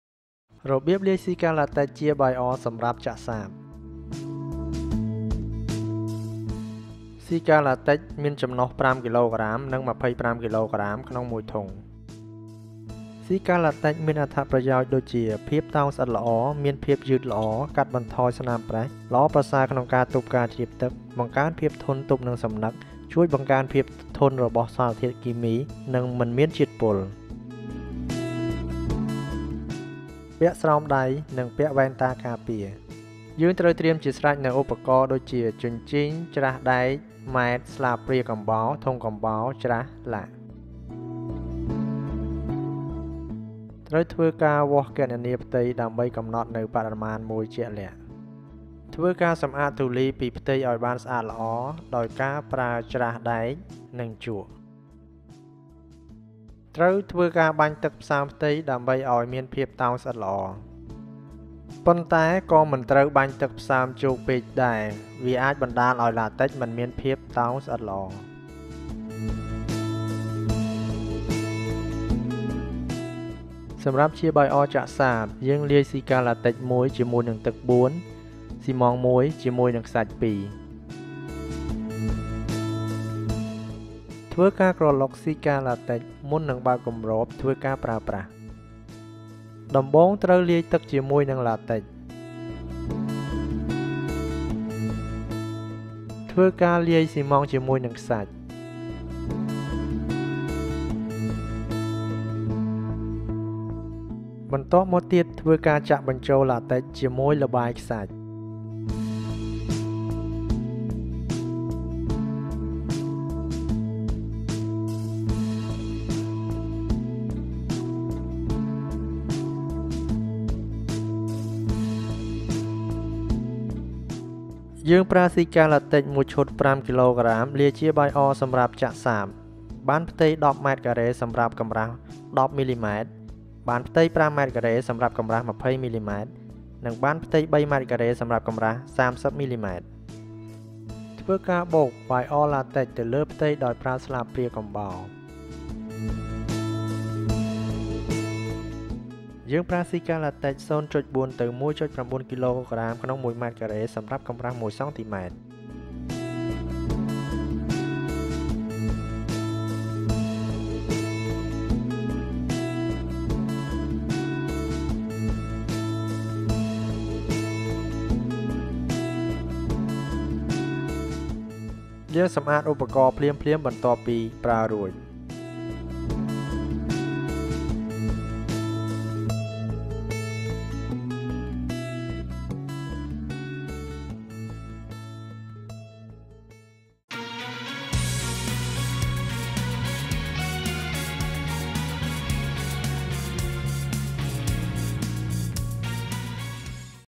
HOW TO APPLY - SikaLatex® AS FLOOR SCREED - ខ្មែរ
0:11 - Packaging
0:13 - Advantages
0:30 - Tools
0:43 - Surface preparation
1:12 - Dosage
1:51 - Mixing
2:02 - Application